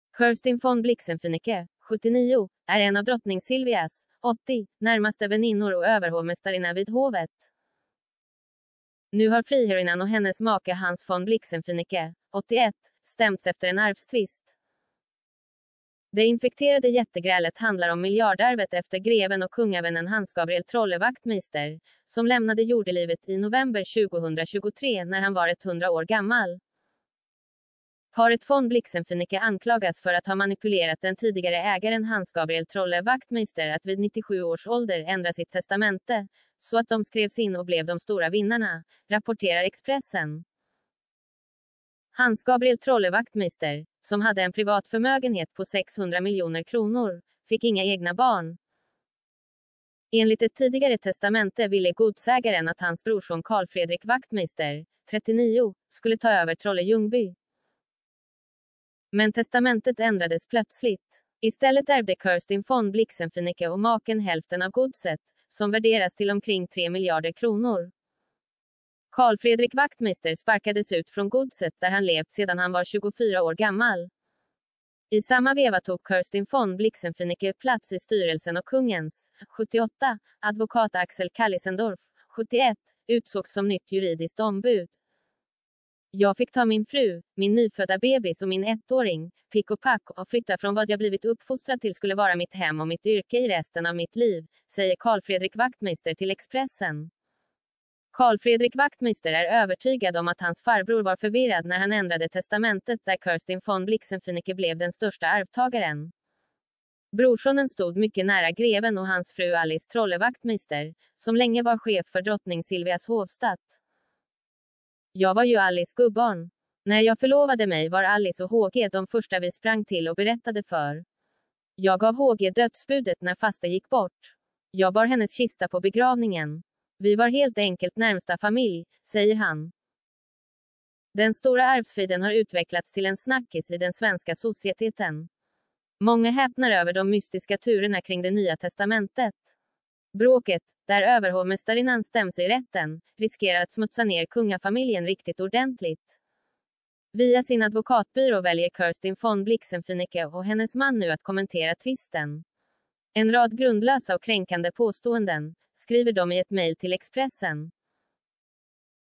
Silvias väninna i miljardbråk – nu stäms adelskvinnan
Drottning Silvias väninna är indragen i ett stort arvsbråk om godset Trolle Ljungby.
Nu stäms Kirstine von Blixen-Finecke och hennes man Hans von Blixen-Finecke i tingsrätten.
Makarna anklagas för att ha manipulerat en förvirrad greve att ändra sitt testamente.